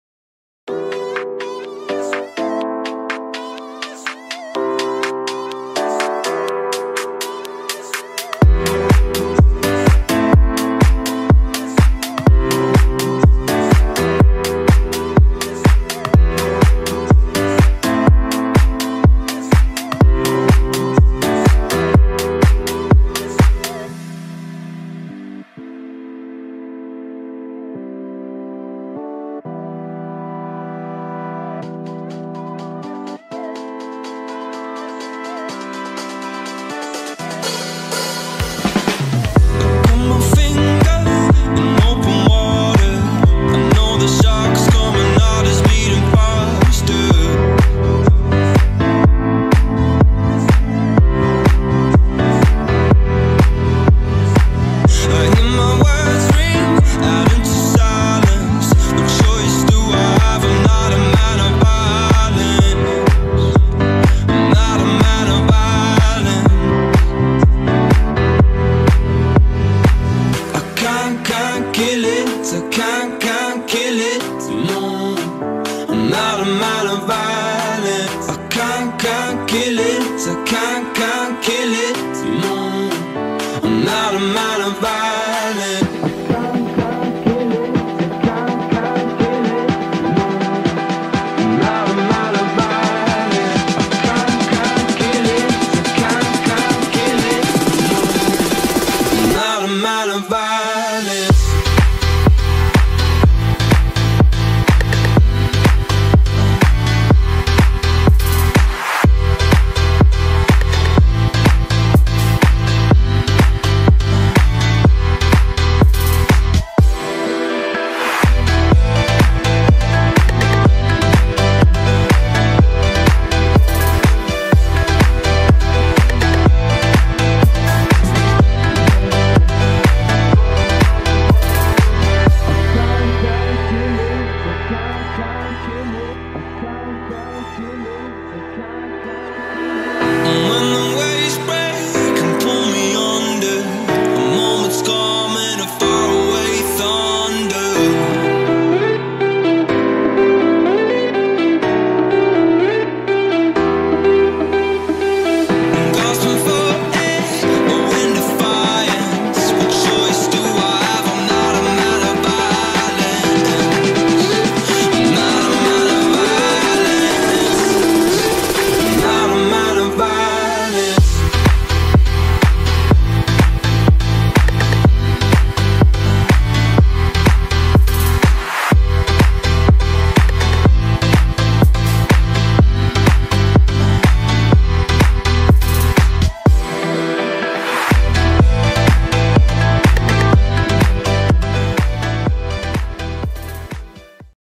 Peter Mazan - Attacking midfielder - Goals & assists
Wideo przedstawia gole i asysty Petera Mazana, 29-letniego pomocnika Radomiaka Radom.
This video shows goals and assist by Peter Mazan, 29 y.o. attacking midfielder currently playing for Radomiak Radom.